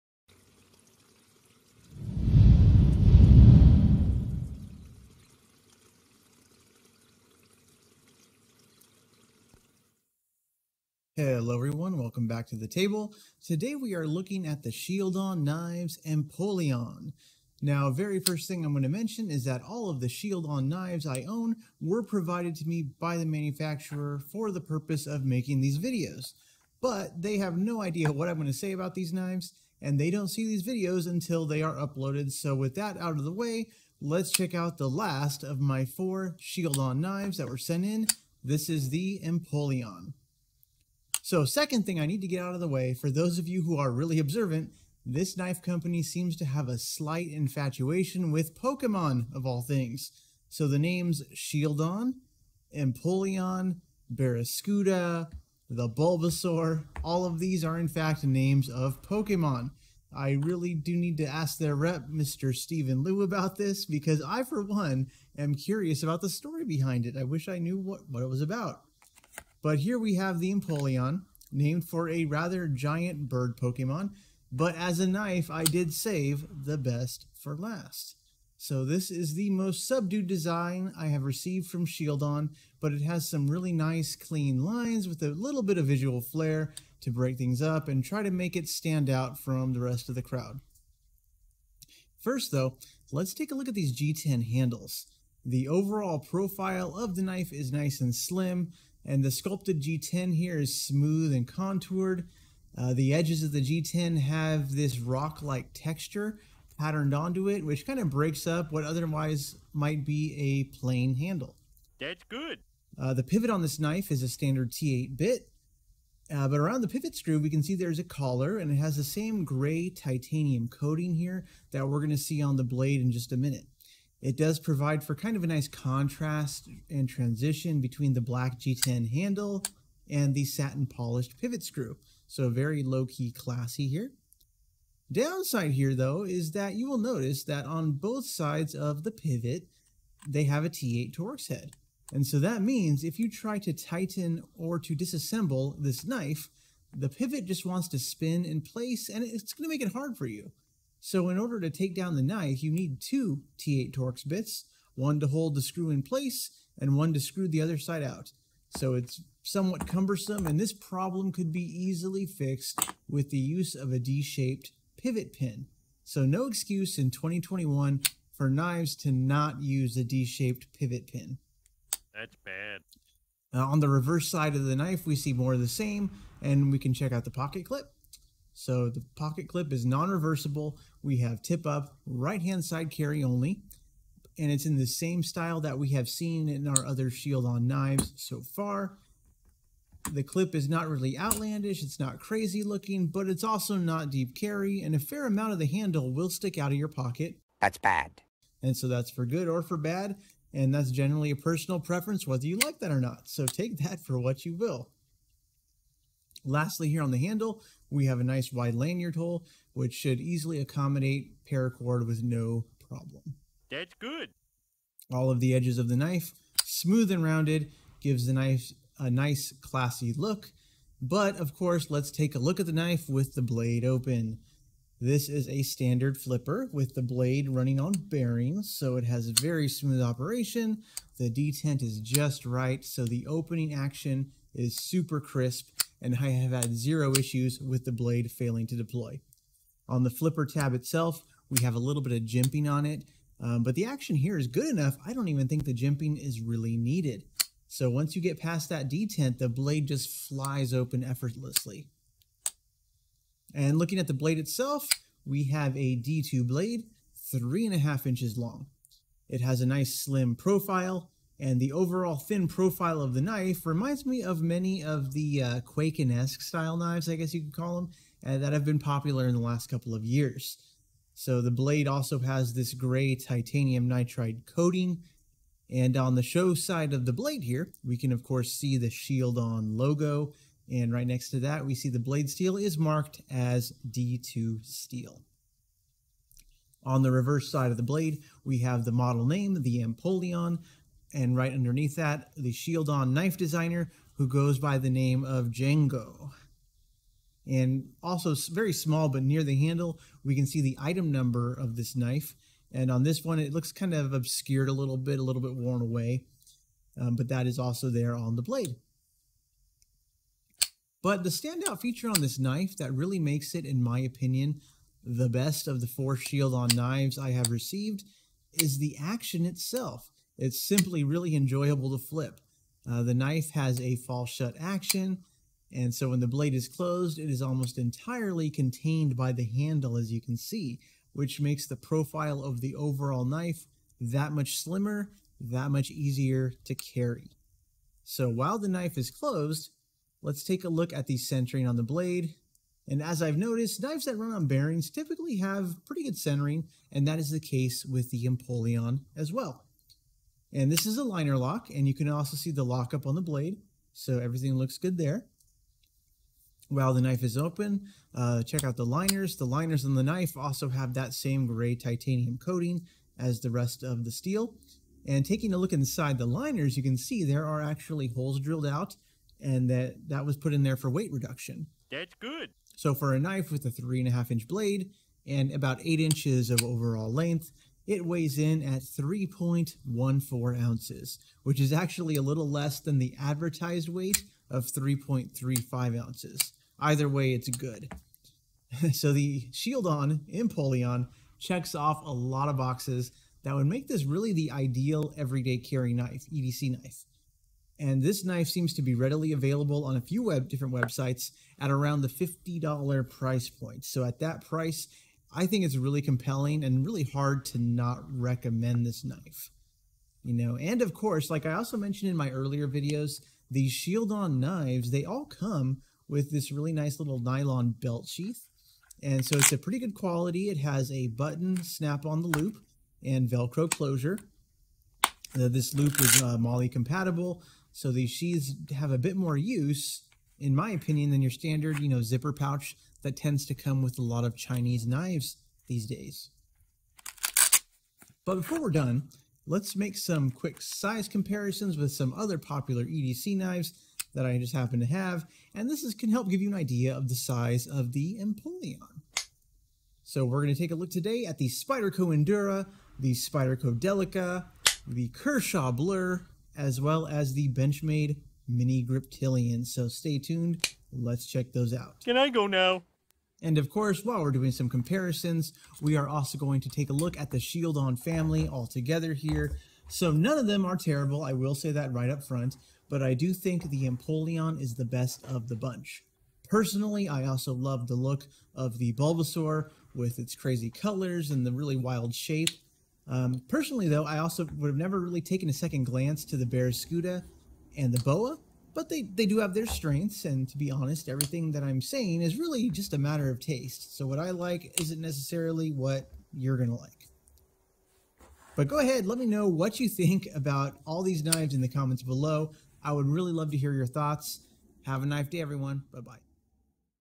The Shieldon Empoleon. Sometimes a simple formula works the best.
Out of my four Shieldon Knives, the simplest design they sent me is actually the least expensive, and the best executed. Check out the Empoleon. And Pokemon?!

LOL I make no money on the links, they just wanted me to pass on the coupon for anyone who wants to buy.